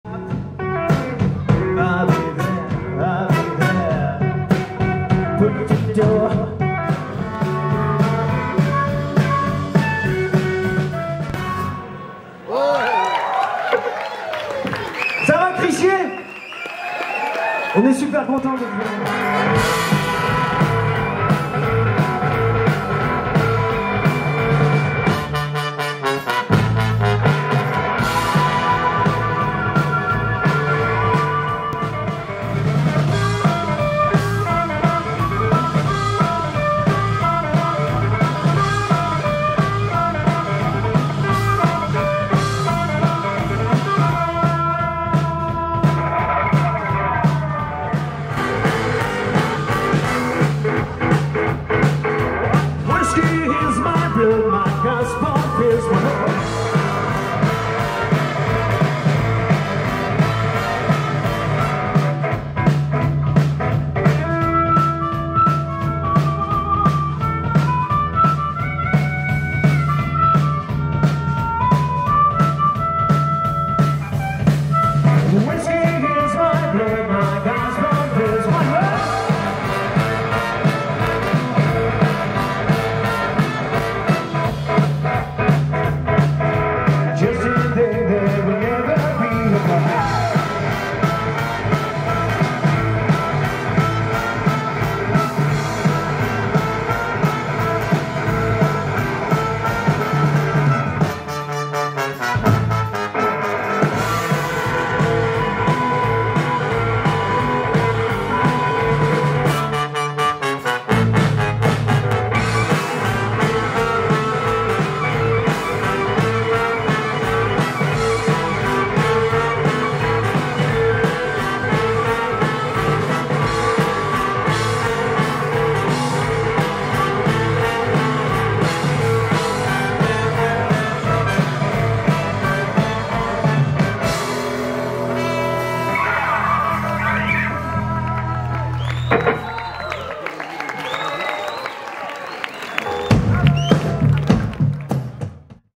🆅🅸🅲🅸🅾🆄🆂 🆂🆃🅴🅴🅻 🅵🆄🅴🅻 🅱🅰🅽🅳
Petite compil du concert de
Vicious Steel Fuel Band
au Crississippi
Blues Rules Festival, Crissier.

Small compilation of the concert of the
at the Crississippi Blues Rules Festival
Crissier. Switzerland.